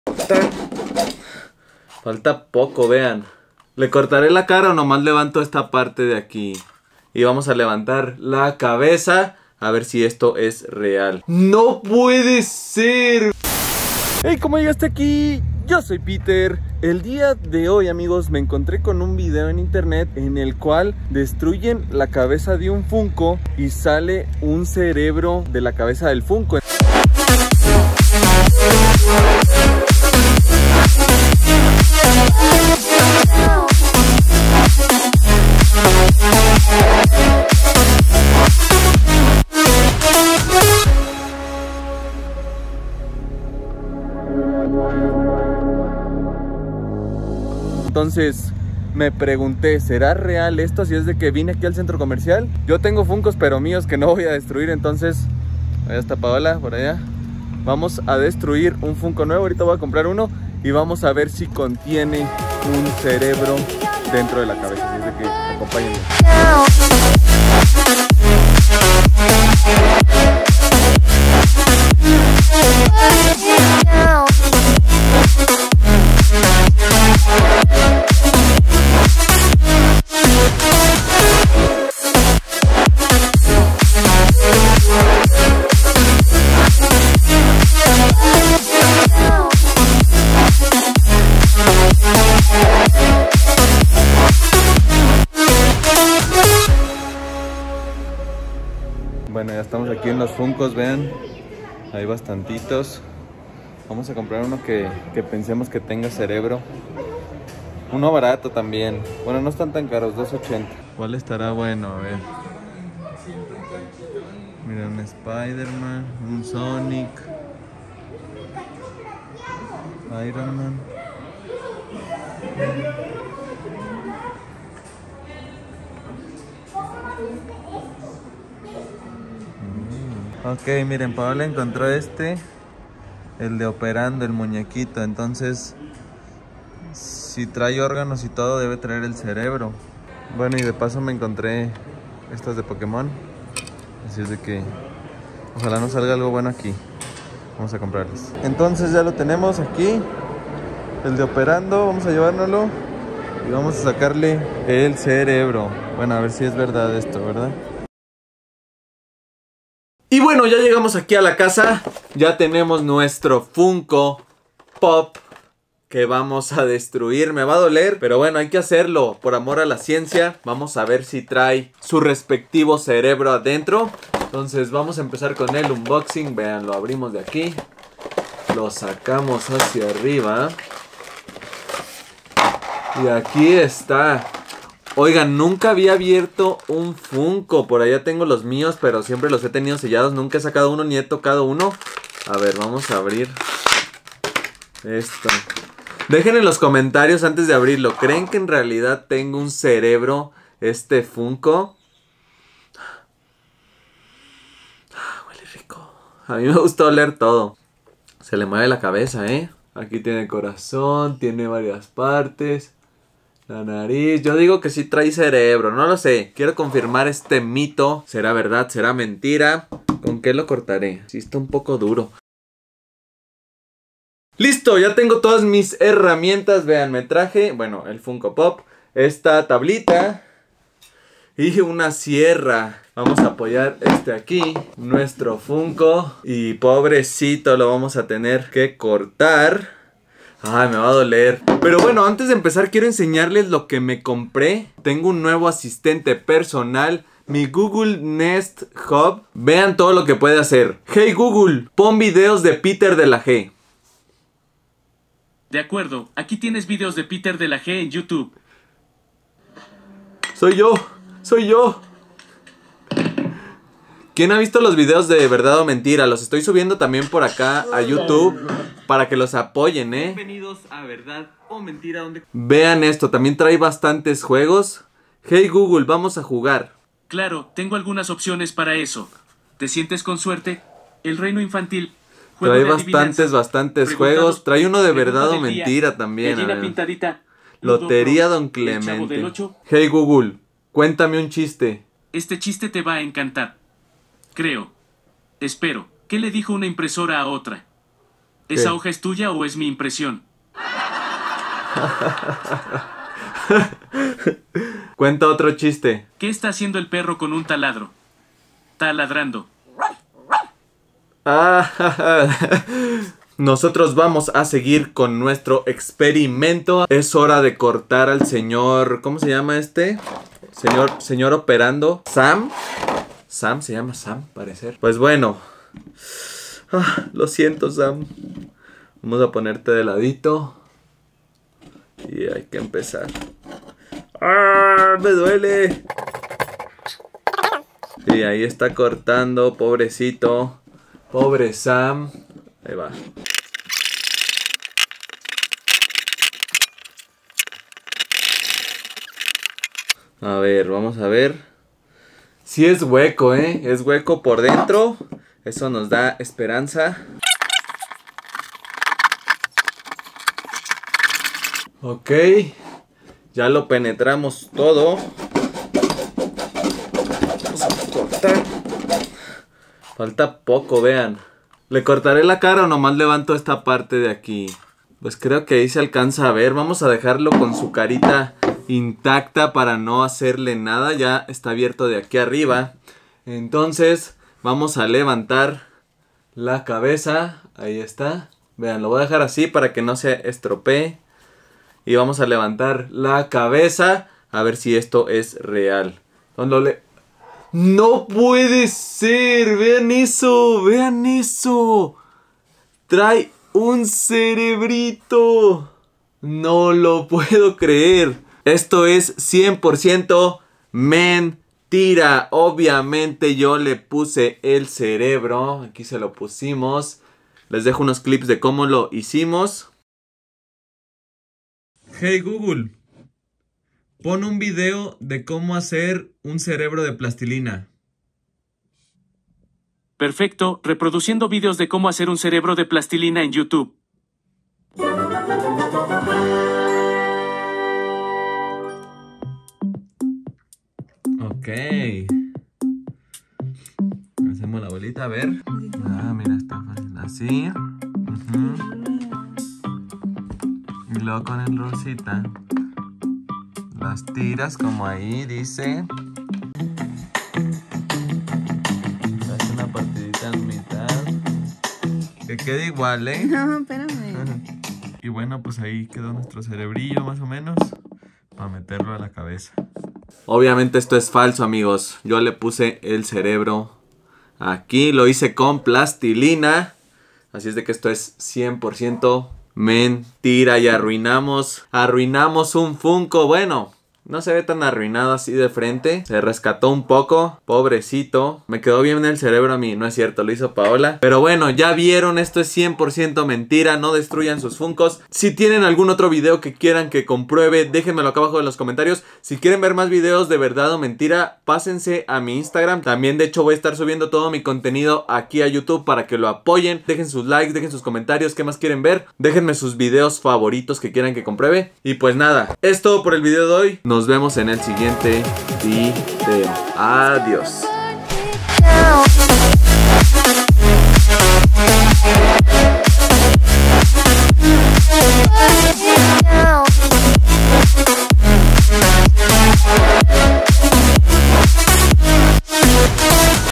ESTO ES LO QUE TIENEN LOS FUNKO POP ADENTRO DE SU CABEZA (Increible)
Dicen que los Funko tienen un cerebro en su cabeza.
Vamos a abrir un Funko completamente a ver que es lo que contiene en su interior.